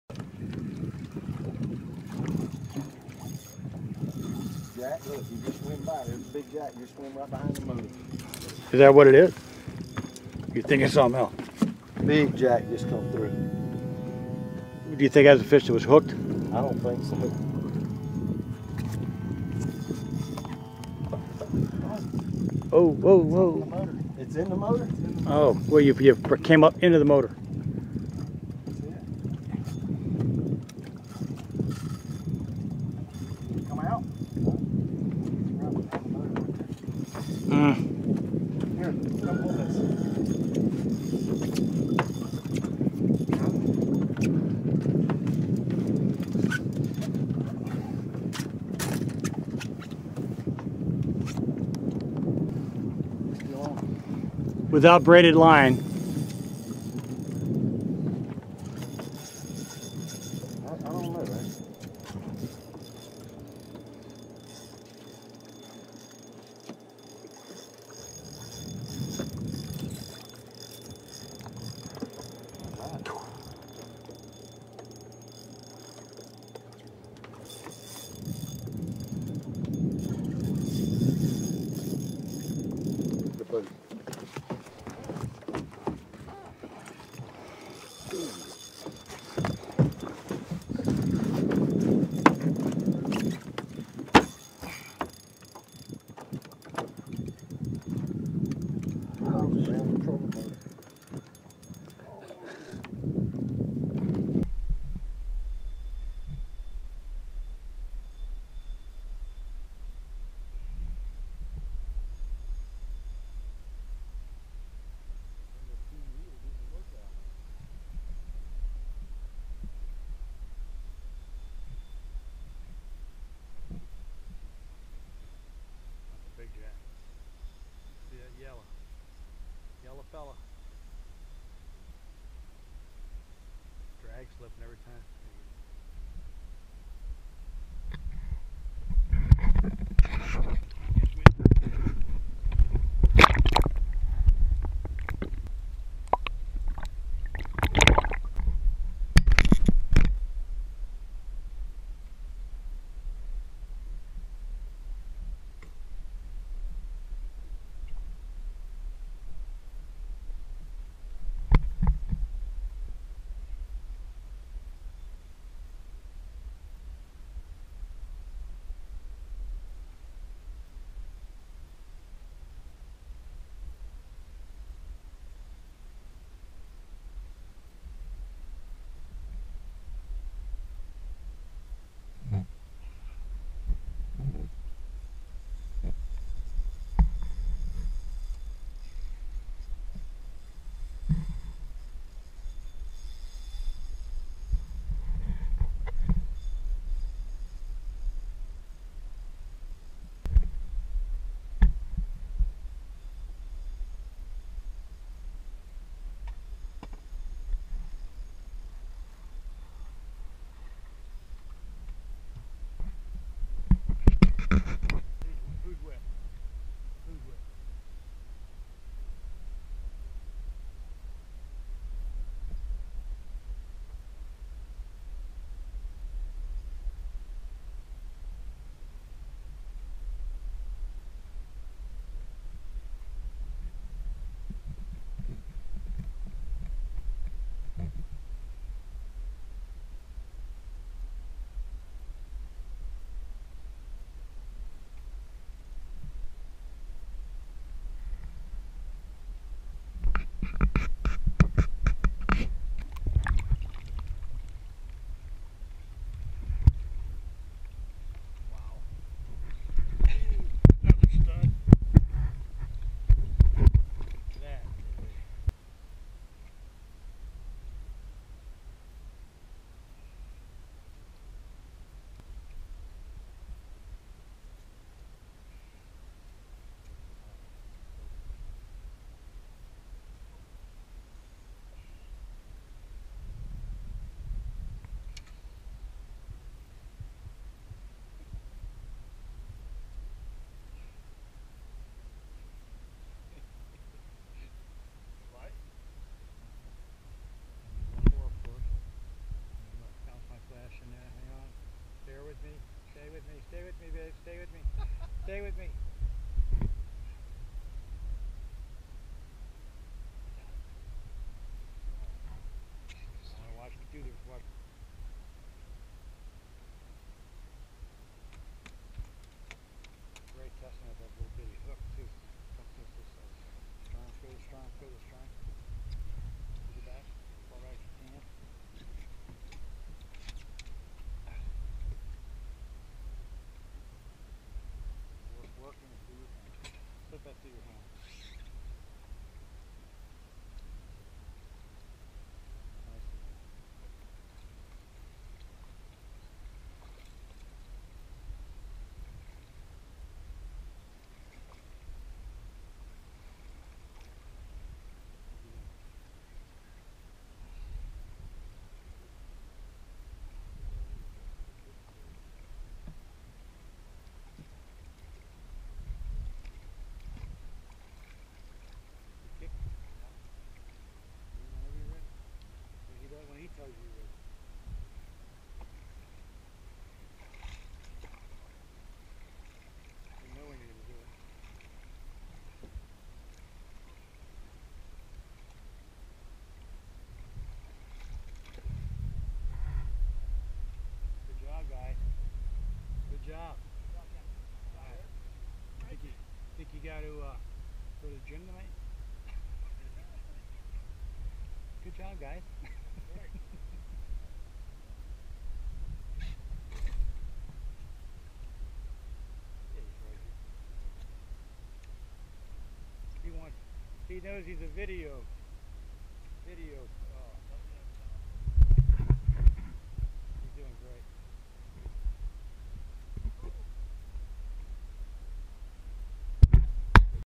Jack Crevalle Wild Light Tackle Rodeo
Few fish fight harder than jack crevalle, and when hooked on tackle intended for use with seatrout or slot redfish, well, things get a little wild, as angler Eric McNally and guide Darien Ladner of Shore Thing Charters learn